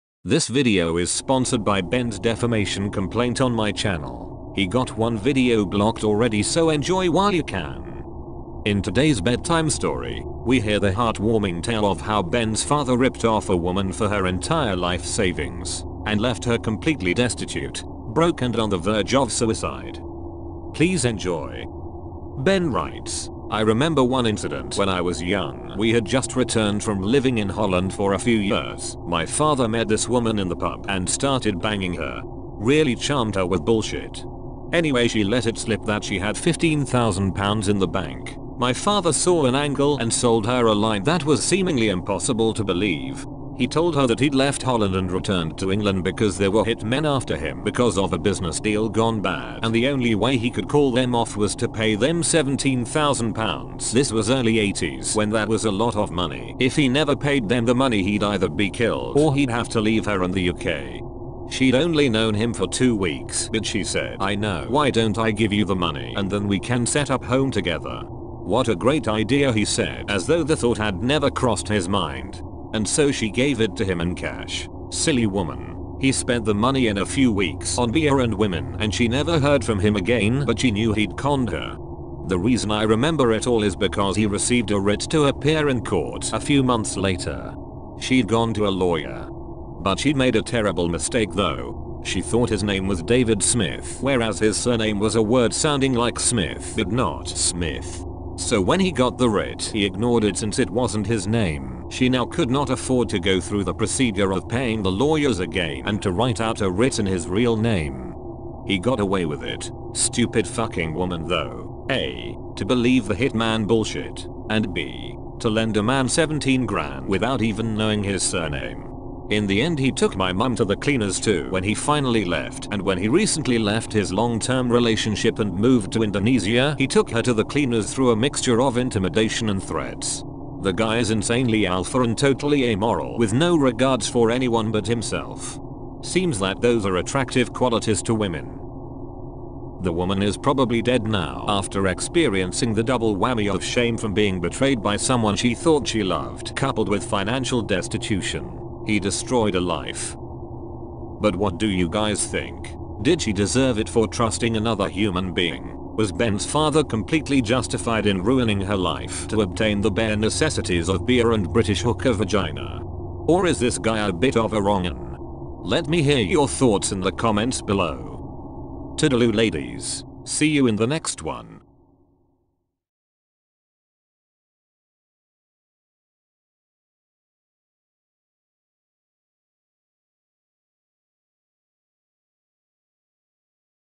BALD AND BANKRUPT steals £17000
Bald & Bankrupt recounts how his father Mr Swift swindled £17,000 and left a woman on the verge of suicide. Preying on the weak seems to be a family tradition. Absolute parasite.
Even pick up artists on the forum thought this guy was a sociopath and called him a scumbag and a mooch.